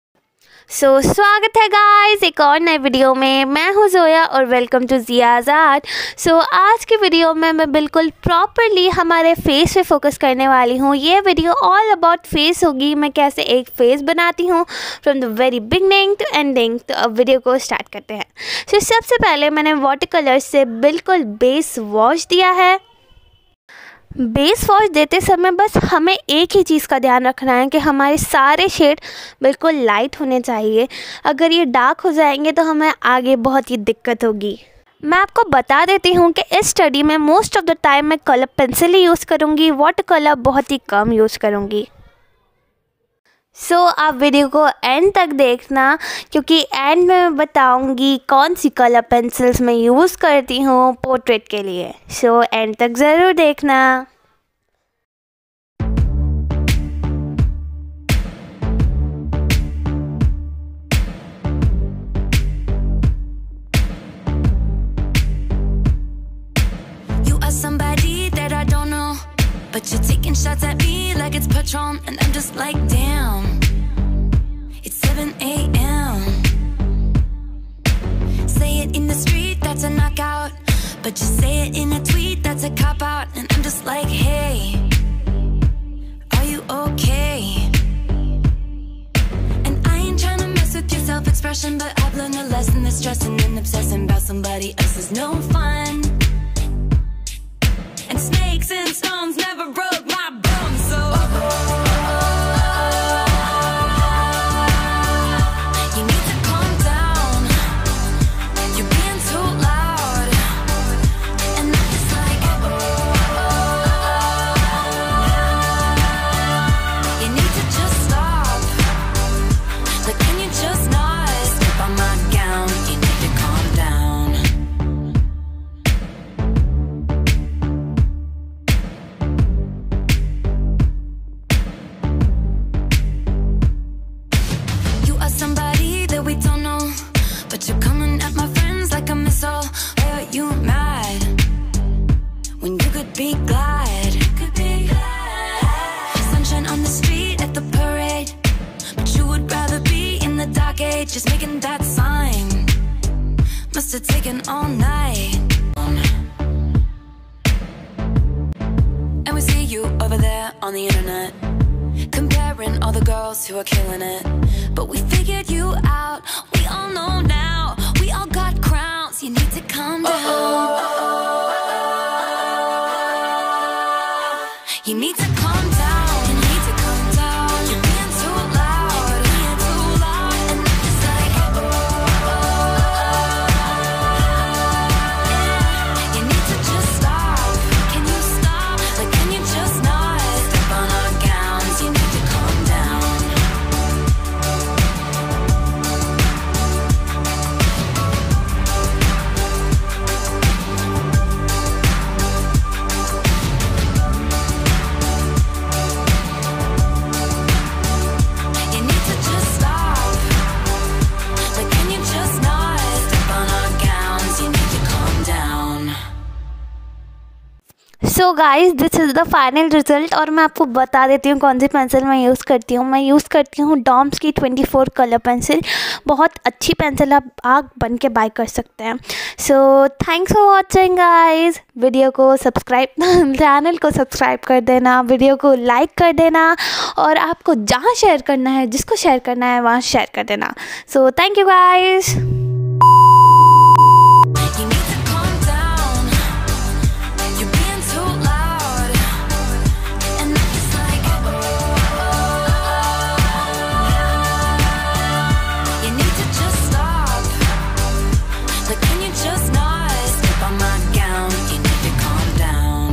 How i work with a face | how draw a face with color pencils| ft watercolors + color pencil|facestudy
How to draw a face in color pencil, ft. watercolor + color pencil

hank u for checking this out
my name is zoya and welcome to zia's art

this video is all about painting a face in watercolor + color pencil , it is a potrait,speed paint .

hope u find it useful

thank u
zia art